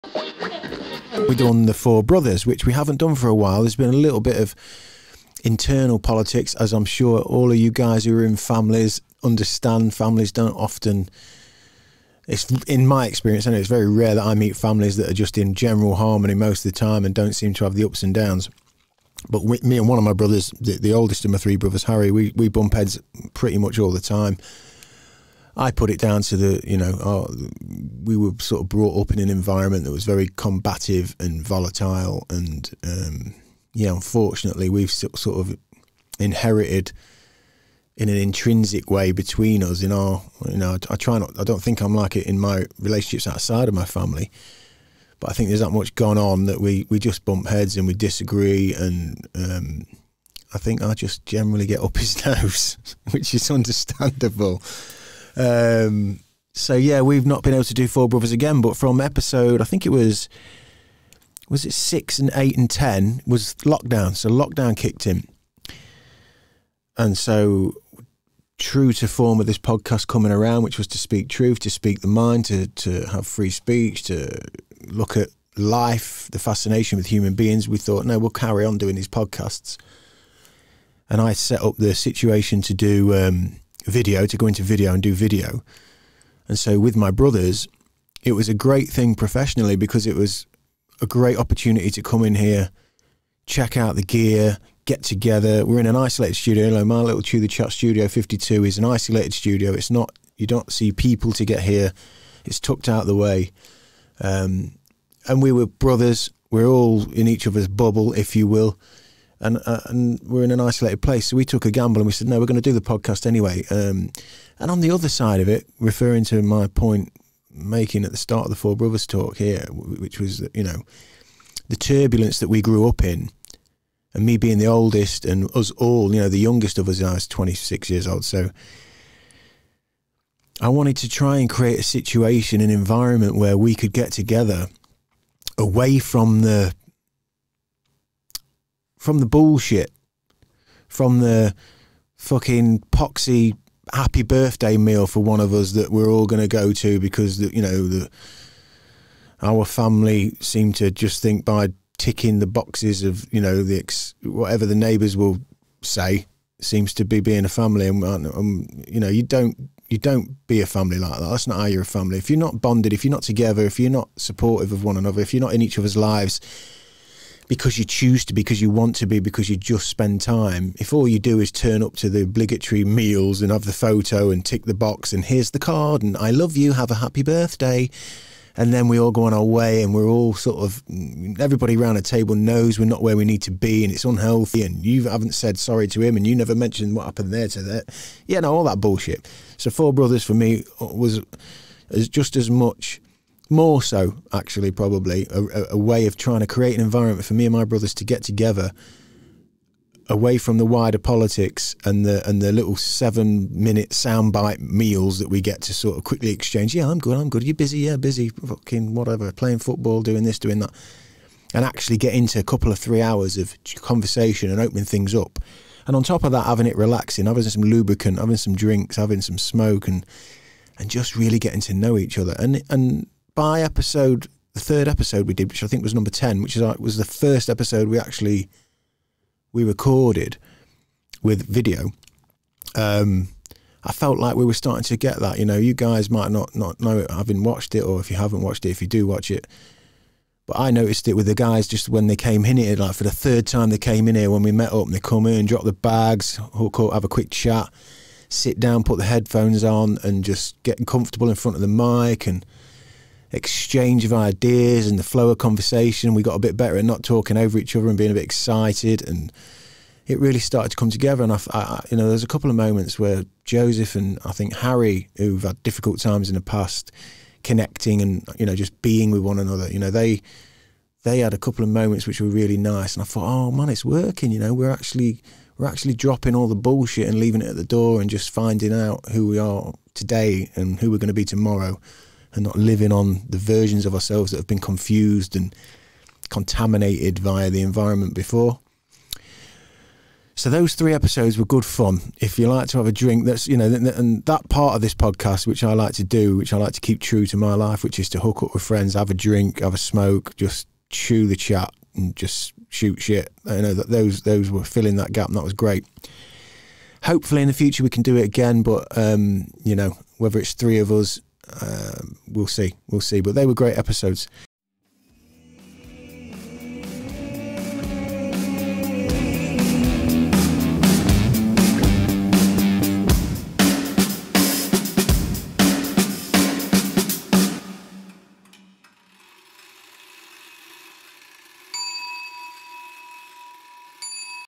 Looking Back At The 4Brothers Episodes | Ep46 Ramble Chat
2020 What A Year Ep46 Ramble Chat

It's a motivational podcast - an inspiring podcast to listen to

Find Out Whats Actually Happening in the Uk:
***UK COLUMN***:

QUESTION — Have a question about topics, stories, appearances, or Anything Else?
Post in comments section of this video!